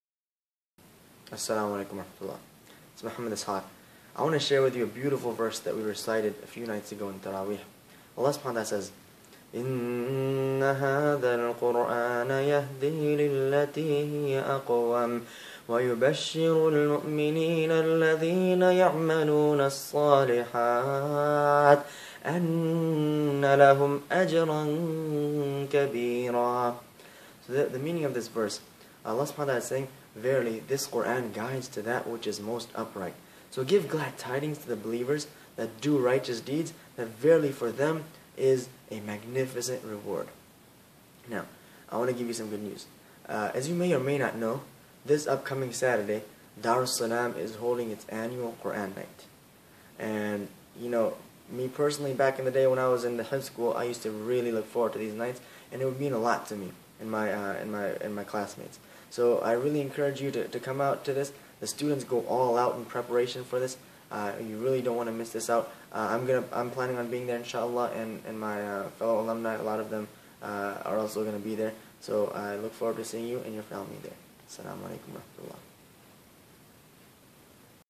[TODAY] Br Muhammad Ishaq - Attend Saturday's Dar-us-Salaam Quran Program Night
[TODAY] Join us for our Quran Program Iftar and Dinner tonight!

We would like to invite you to join Br Aamer Khan, Dr. Altaf Husain and others at this Saturday's Quran Program Night Iftar and Dinner at the Ritchie Coliseum at the University of Maryland College Park. Br Aamer Khan was one of our first graduates at the Dar-us-Salaam Hifzh School and is currently pursuing a career as a medical doctor and will be joining us as well as Dr Altaf Husain! Dr Altaf Husain is a nationally recognized Muslim Leader and Speaker and supporter of Quranic Education!

From the Full Time Hifzh school, to the Quran Institutes located around the DMV region, Saturday night will be a program to celebrate our Quran programs.

We all look forward to seeing you this Saturday!